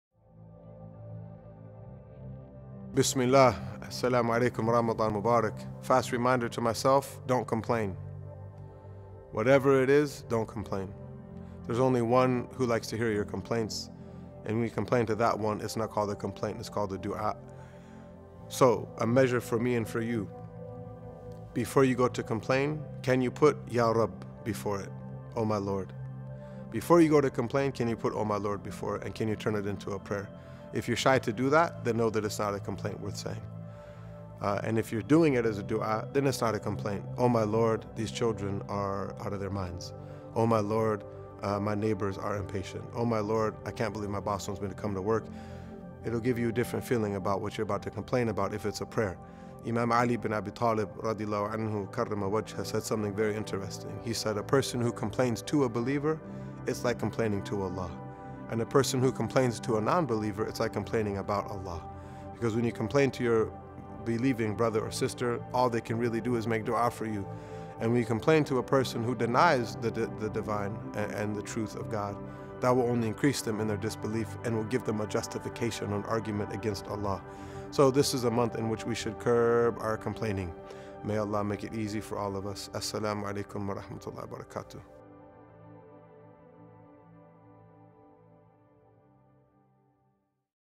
FAST REMINDERS :: Don't Complain
Ramadan is a time in which we should curb our complaining, and instead make it a prayer.

Please keep Ta'leef in your prayers and support our Ramadan 2016 Campaign: "Ta'leef Together" to raise our goal of $250,000. or text taleeframadan to 41444.